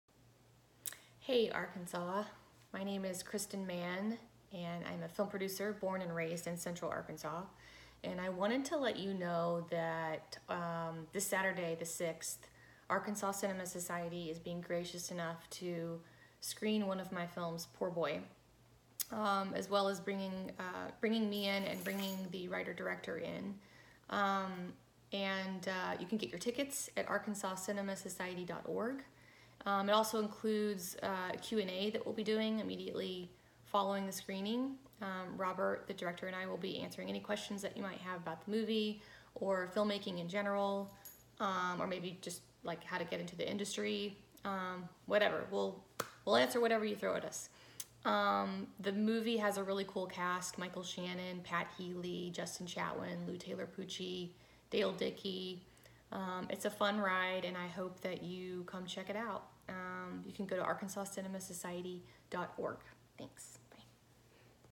Kristin Mann talks about Saturday's Screening Poor Boy
Come find out why Kristin says this film is a wild ride.